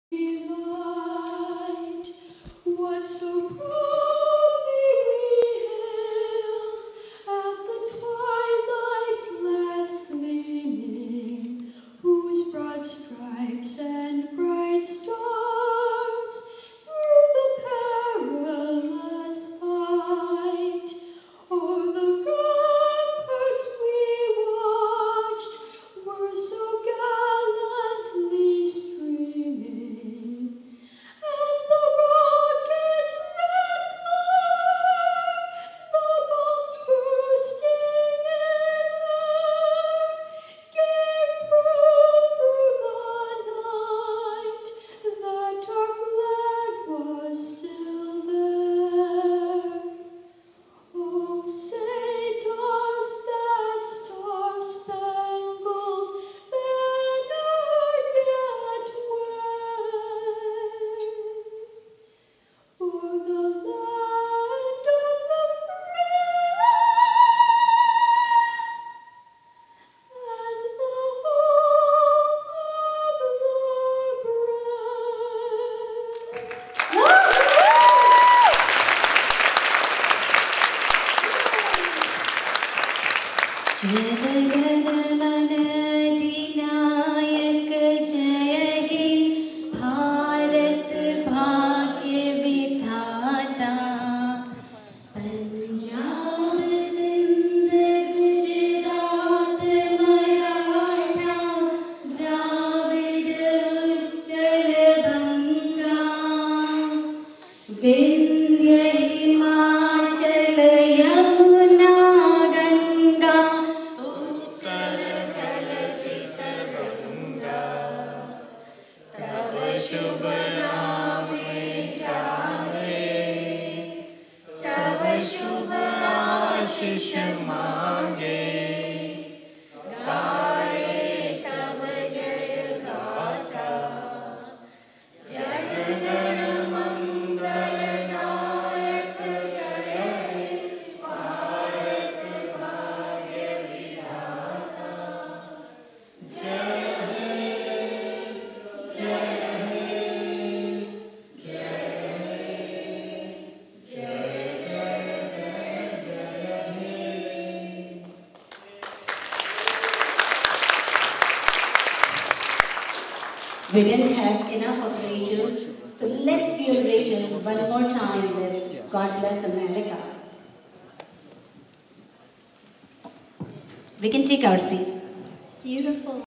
Gift of Life USA - Annual Fund Raising Gala 2018 Part 3
Gift of Life USA - Non-Profit Organization For Special Needs --Annual Fund Raising Gala 2018 at Huntington Hilton, NY - Part 3